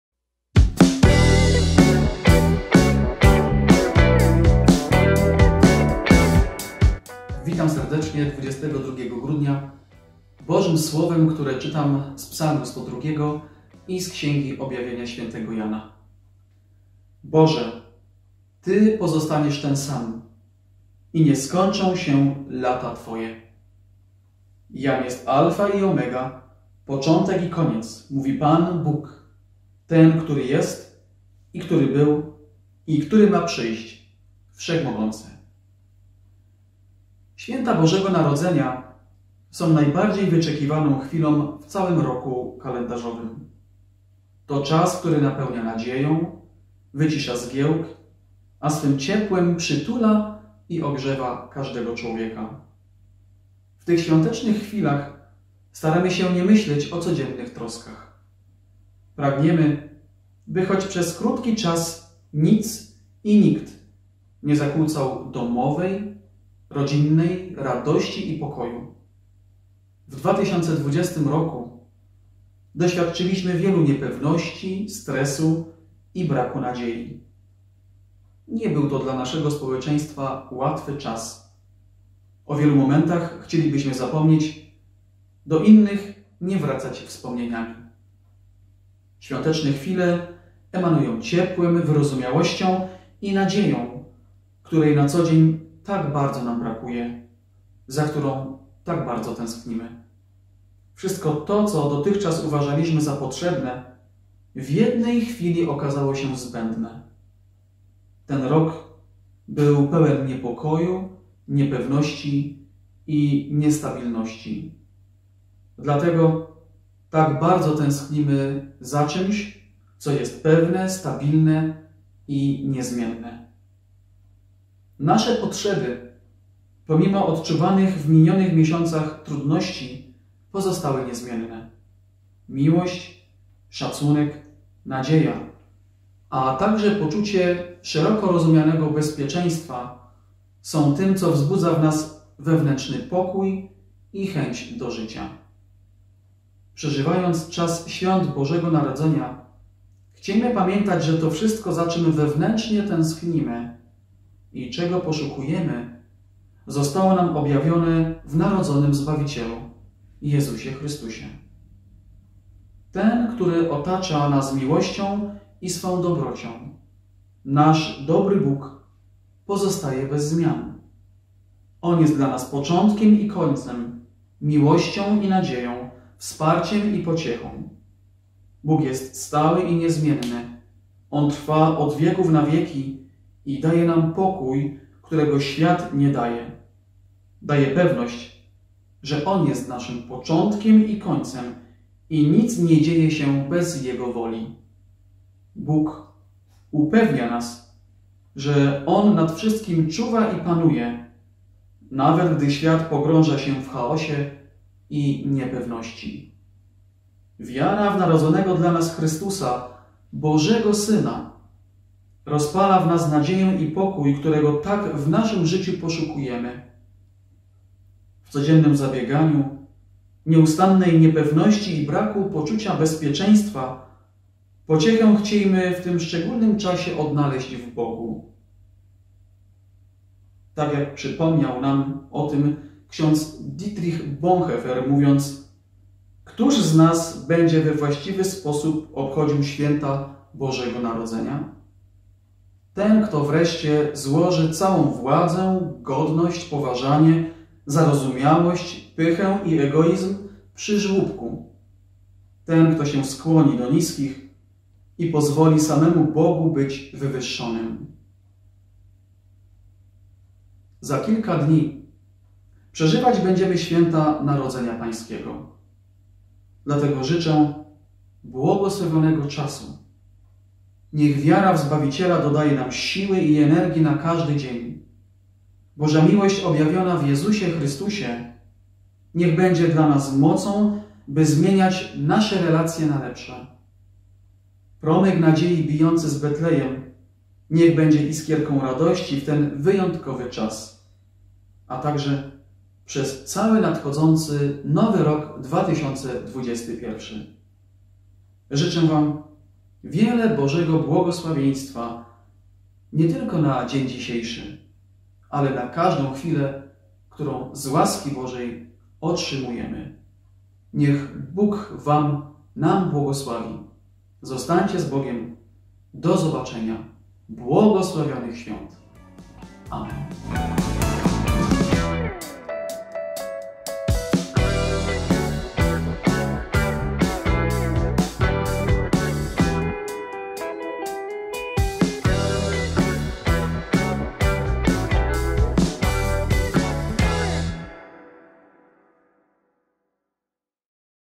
Codziennik biblijny, Słowo na dzień 22 grudnia 2020 r.
ks. Mateusz Łaciak, Parafia Ewangelicko-Augsburska w Kluczborku
Rozważanie biblijne na Słowo dnia:

Boże, ty pozostaniesz ten sam i nie skończą się lata twoje.

Jam jest alfa i omega (początek i koniec), mówi Pan, Bóg, Ten, który jest i który był, i który ma przyjść, Wszechmogący.
Objawienie Jana 1,8
Codziennie o 7 rano spotkanie z Biblią.